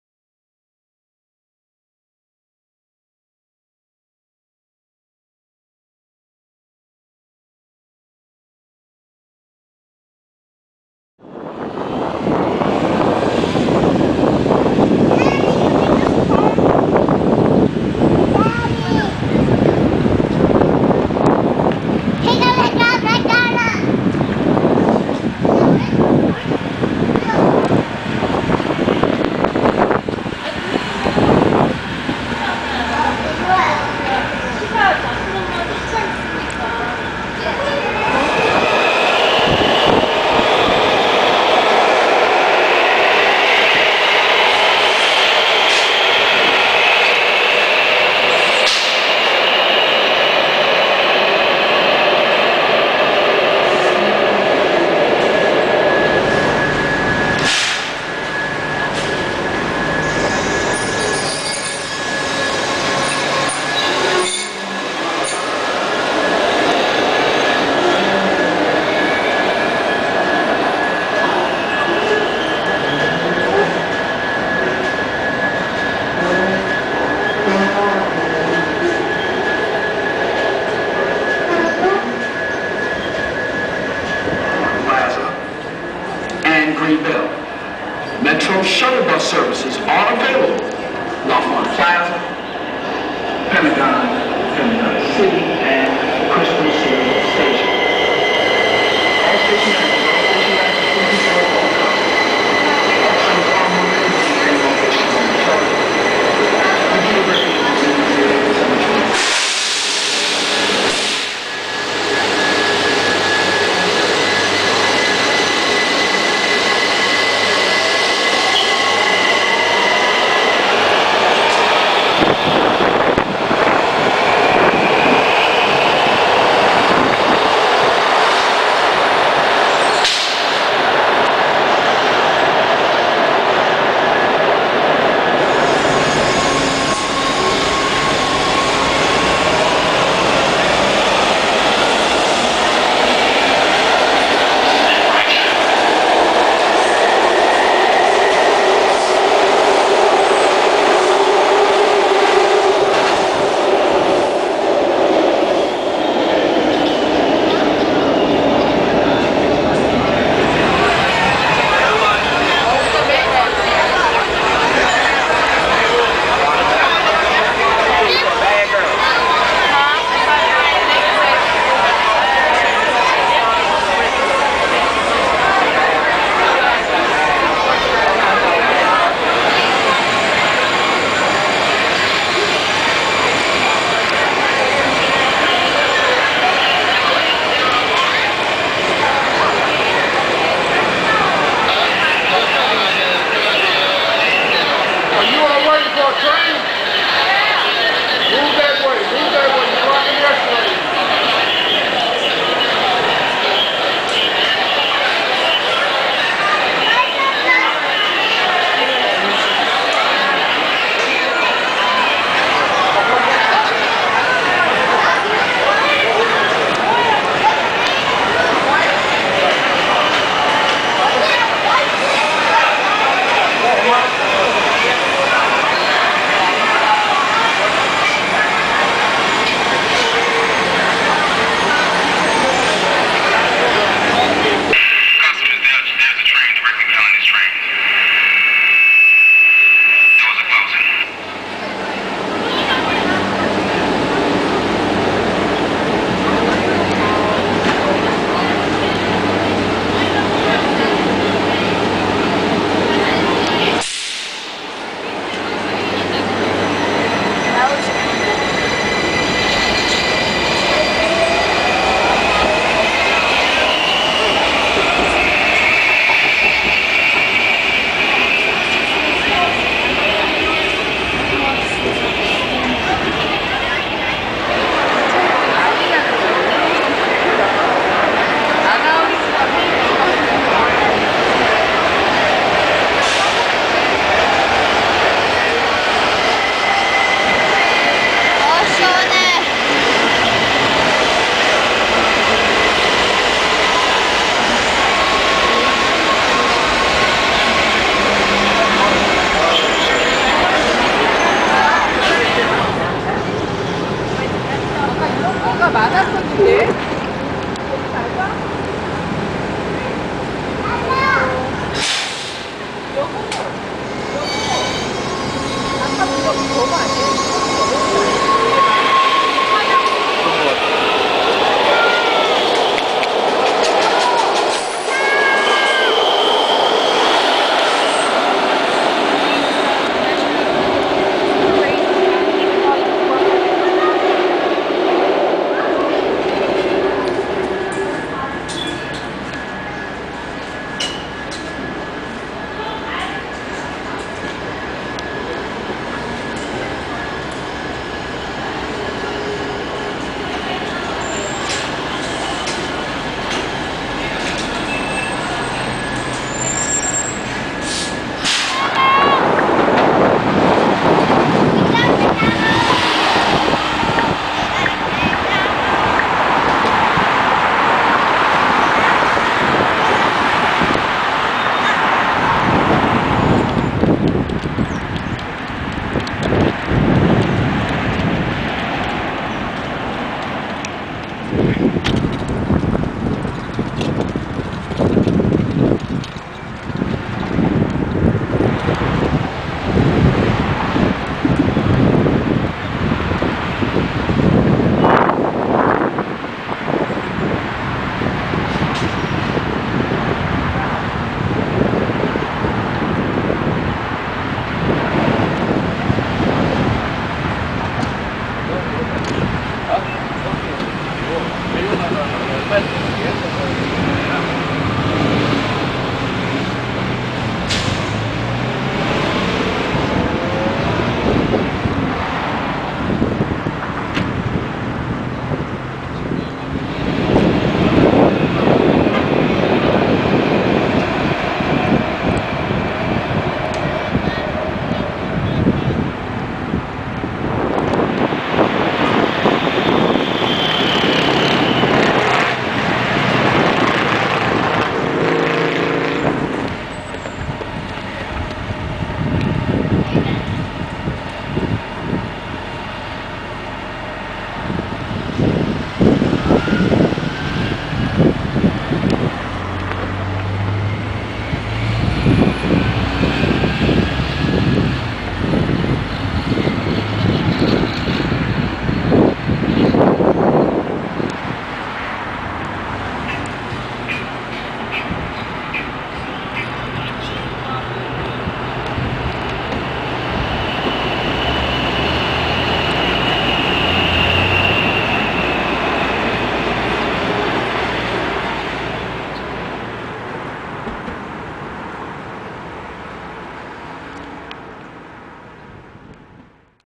April 3 & 4, 2010 Buffing - WMATA Metrorail/Metrobus (featureing 1000/4000/5000-series) & Ride-On
Here are various videos taken between April 3, 2010 & April 4, 2010, featuring the WMATA Metrorail system, and a bit of WMATA Metrobus and Ride-On (Montgomery County Transit)! Retired WMATA 1000-series, WMATA 4000-series, and WMATA 5000-series cars can be seen running in service.